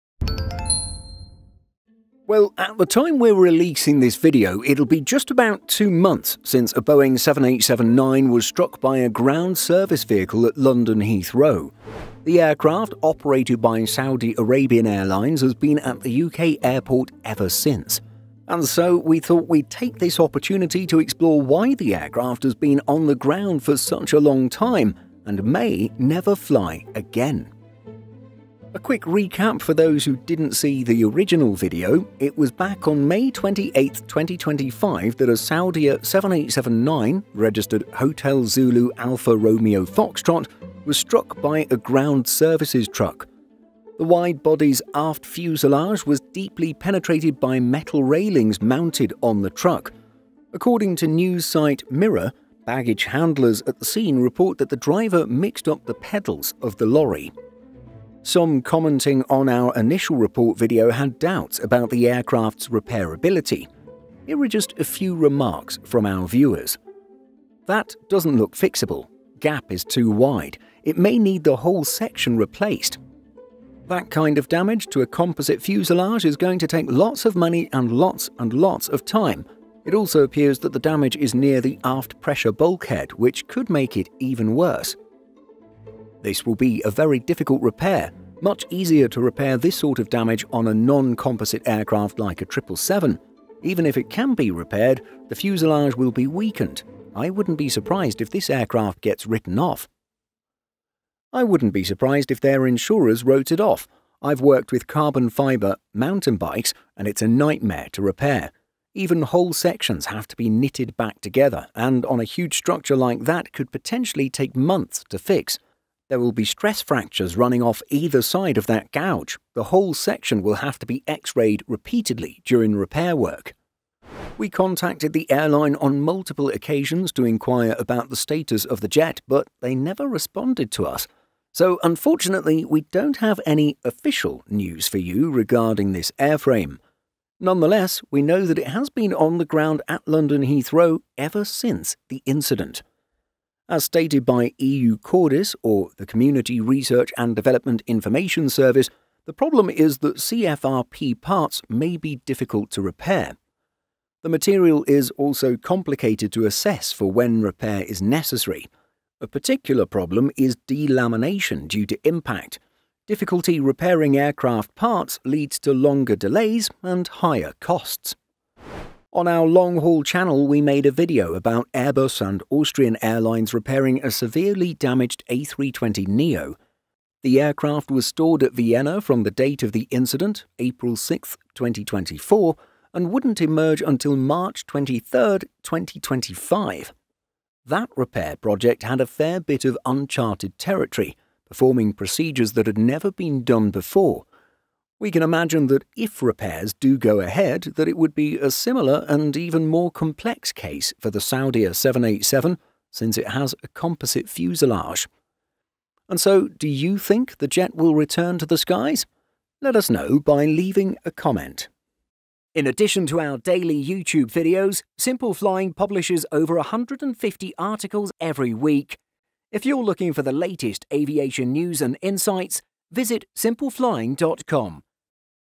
Will This Boeing 787, Damaged By Ground Vehicle, Ever Fly Again?
Well, at the time we’re releasing this video, it will be just about two months since a Boeing 787-9 was struck by a ground service vehicle at London Heathrow. The aircraft, operated by Saudi Arabian Airlines, has been at the UK airport ever since.

And so we thought we’d take this opportunity to explore why the aircraft has been on the ground for such a long time and may never fly again…